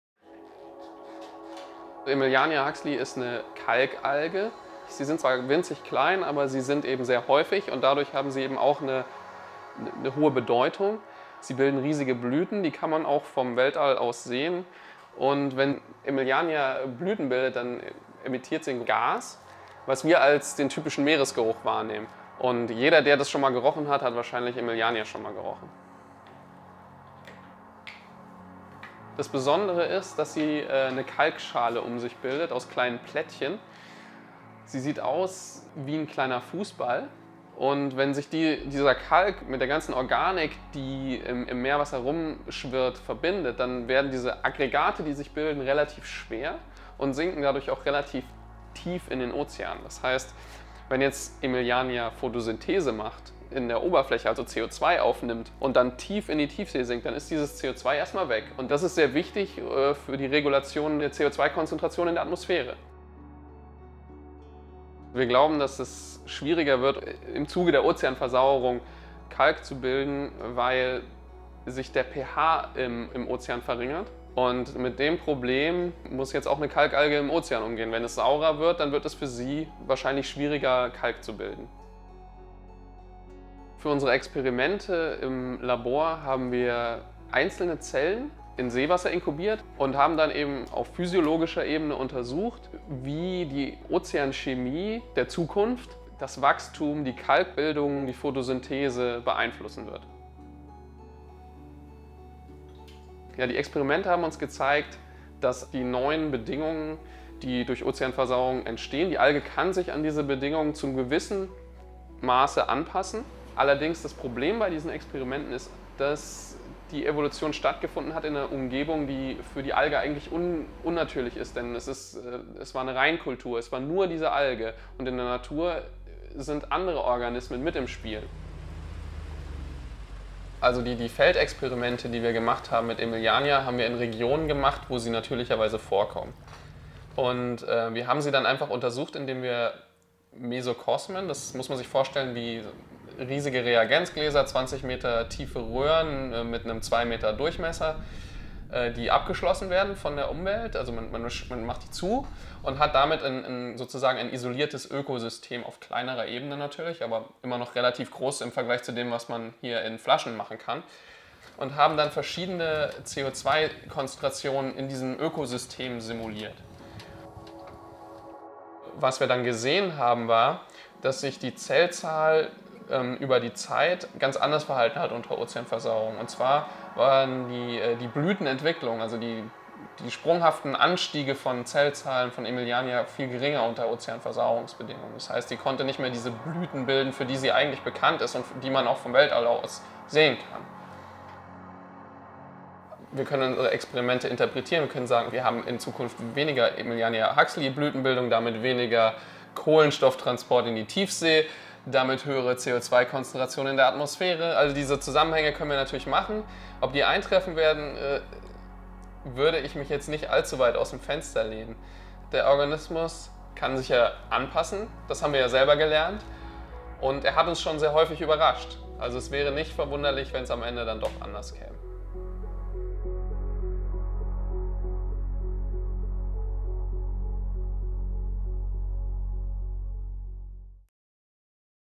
BIOACID Science Portrait: Lennart Bach
Lennart Bach, marine biologist at GEOMAR Helmholtz Centre for Ocean Research Kiel investigates a tiny organism that can hardly be seen with the naked eye – and is still visible from space. The single-celled calcifying alga that his research focuses on is called Emiliania huxleyi. It is a true multi-talent: Its calcium carbonate platelets carry organic material from the surface to the deep ocean, which regulates carbon dioxide concentrations in the atmosphere. A gas that it releases – and that we perceive as the typical sea smell – is thought to serve as cooling agent in the climate system.

Experiments revealed that the alga is able to adapt to ocean acidification through evolution to a certain degree. But in its natural environment, it fails to prevail. But nonetheless Lennart Bach thinks: “The organism has frequently surprised us. So it would not be entirely astonishing if there was another twist in the plot.”

The BIOACID science portraits complement a photo exhibition on ocean acidification is funded by the German Federal Ministry of Education and Research (BMBF) as part of the Science Year 2016*17 – Seas and Oceans.

Maarten van Rouveroy van Nieuwaal

Producer
Maike Nicolai

Additional Footage
Jacques Descloitres & Jeff schmaltz, MODIS Rapid Response Team, NASA/GSFC
Christian Howe, Submaris
Maike Nicolai, GEOMAR
SEM Lab, Institute of Geosciences, Kiel University

BIOACID 2017